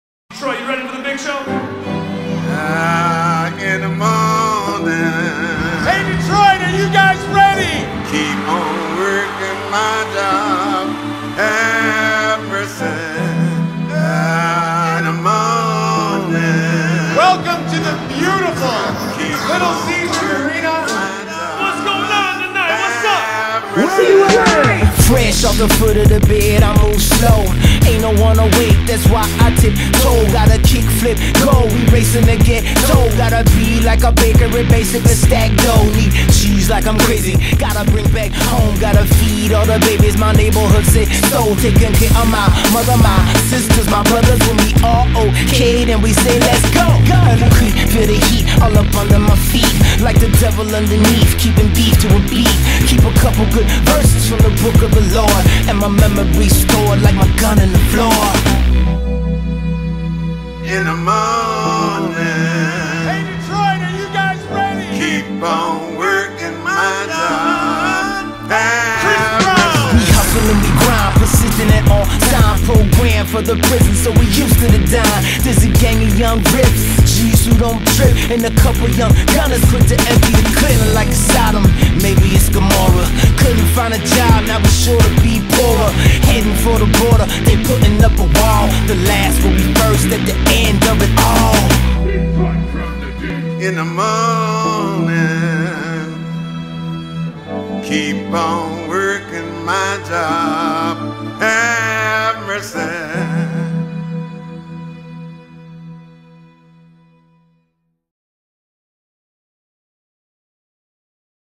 2017 BIG SHOW | Chris Brown, Jeezy, NF, Tee Grizzley
Channel 955 and 97.9 WJLB present “Big Show 2017” featuring Chris Brown, Jeezy, Tee Grizzley, NF, Trick Trick, Polo Frost and more at Little Caesars Arena in Detroit, Michigan.